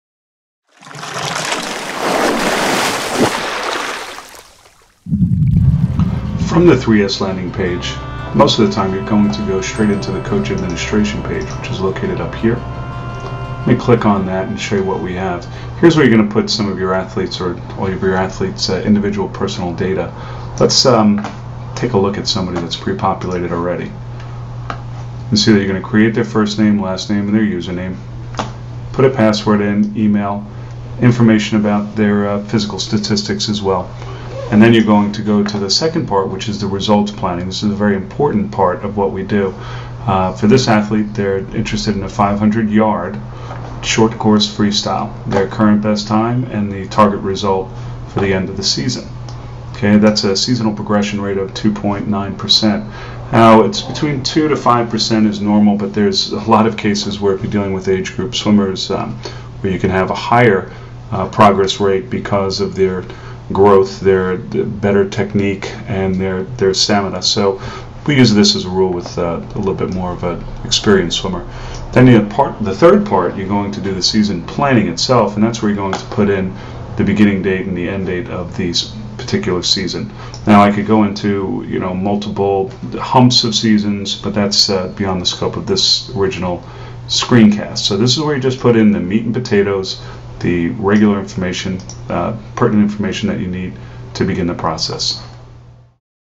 Swimming Seasonal Training Design Tools # 2
Learn how to utilize the Super Sport Systems suite of online seasonal design tools in this series of online tutorials. This is the second in the series. Best viewed full screen in 1080p